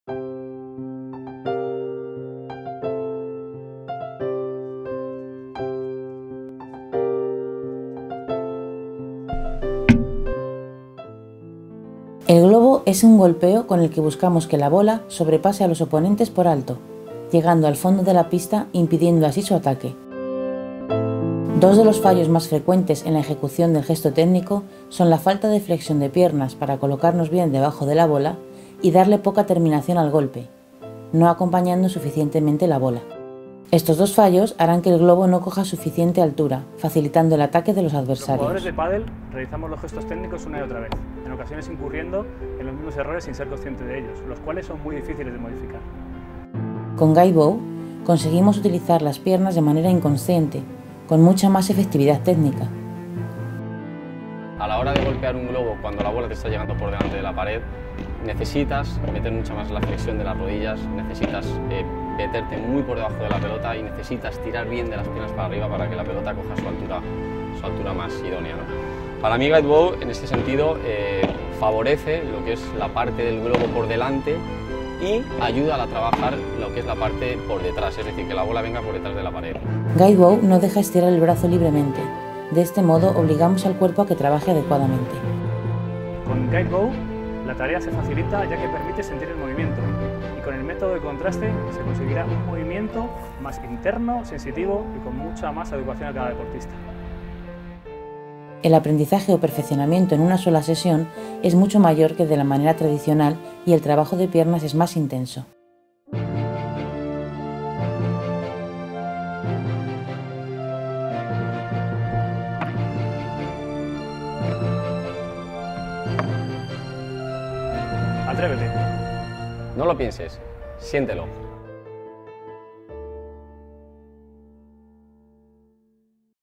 Globo Pádel
El globo es un golpe con el que buscamos que la bola sobrepase a los oponentes por alto, llegando al fondo de la pista impidiendo así su ataque. Guidebow ayuda en una sola sesión a que el jugador aprenda a golpear  la bola, utilizando sus piernas y haciendo una parábola más alta y con más profundidad.